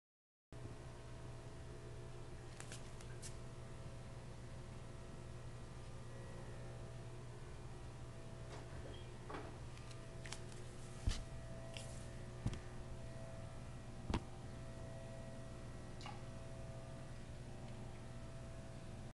pet largemouth bass eating worm
my pet largemouth bass eating a worm